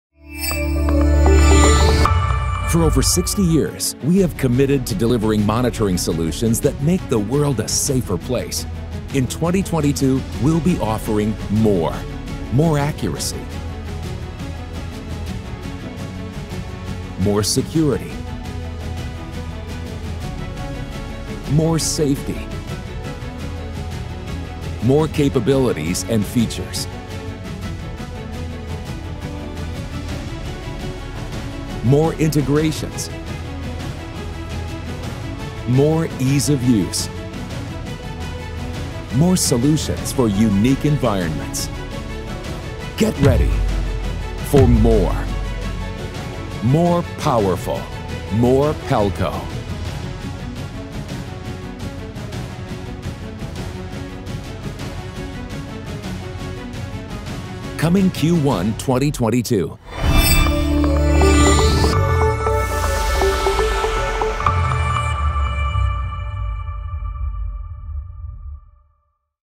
A New Era of Video Security by Pelco
Our new Sarix Modular, Sarix Corner, Sarix Multi, Spectra Enhanced 7 IR Look Up and Body-Worn Cameras, deliver enhanced accuracy, smart integrations and improved safety and security for organizations all over the world.

Connect with Pelco!